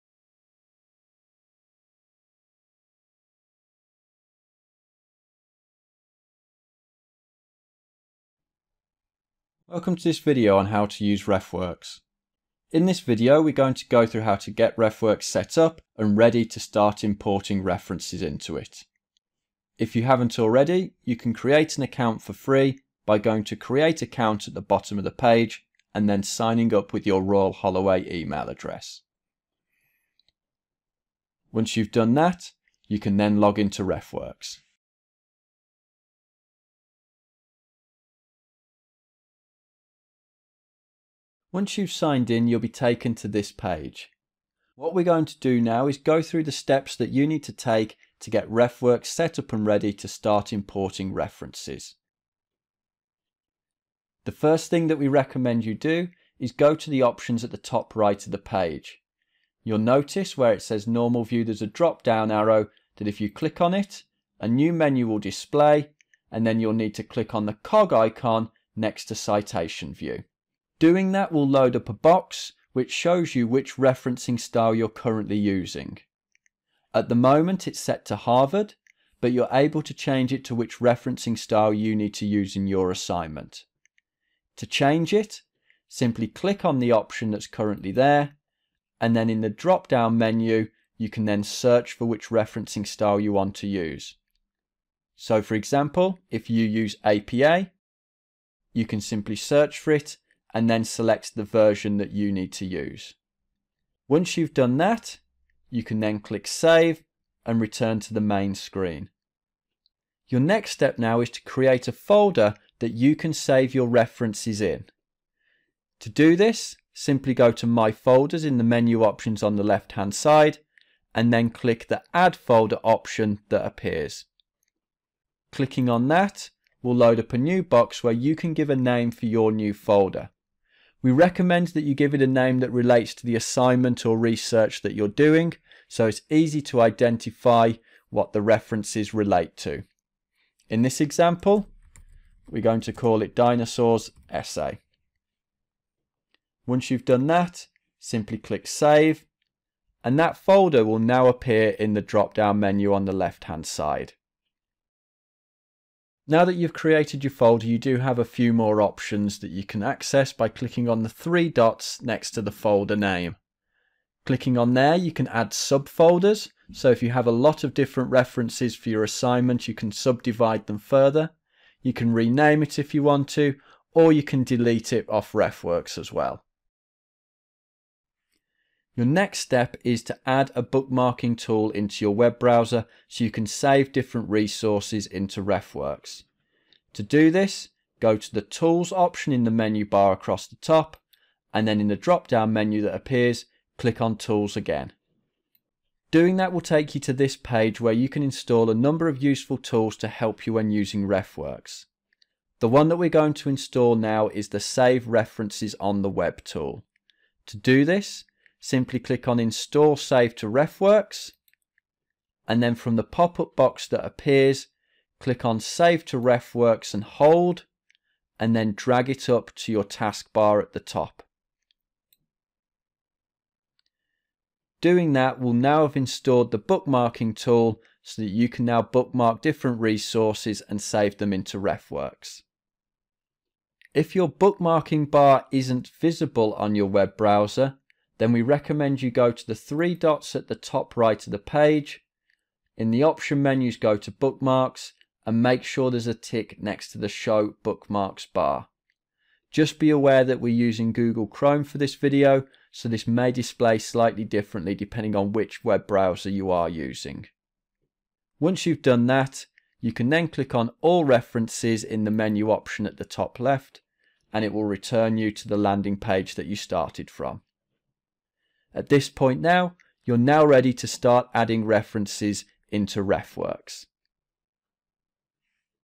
RefWorks Part 1 - How to set up RefWorks
Description:
Part 1/5. This video shows Royal Holloway Library users how to create an account and set up RefWorks so you can start importing references.